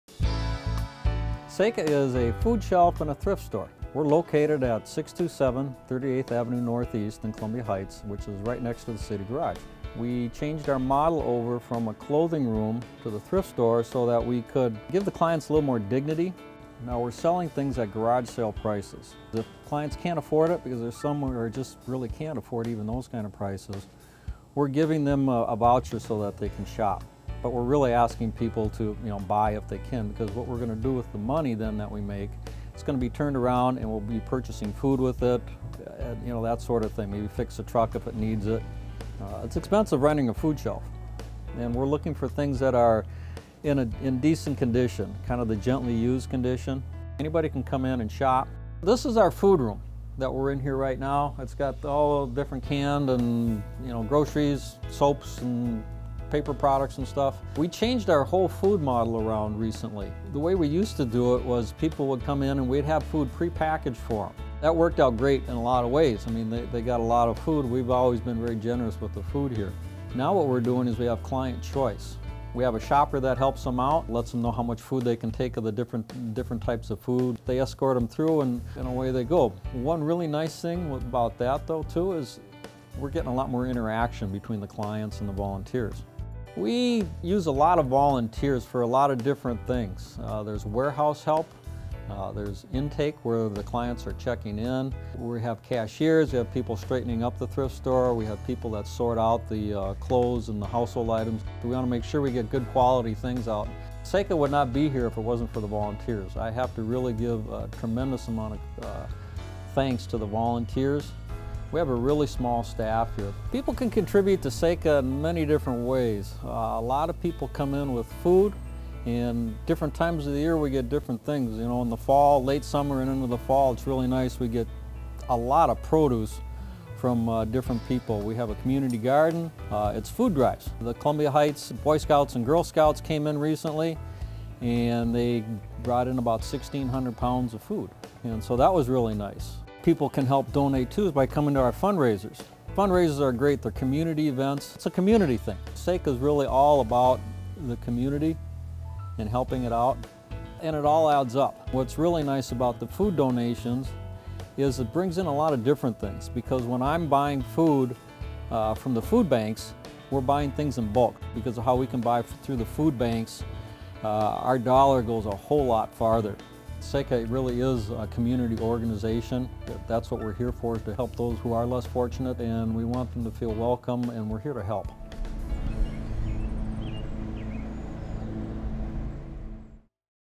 SACA - 2014
Dave Rudolph, Manager of SACA (Southern Anoka County Assistance) located at 627 38th Ave. NE in Columbia Heights provides viewers with a walking tour of the newly revised center that now includes a thrift store. He talks about volunteer participation and how you can donate to the food shelves.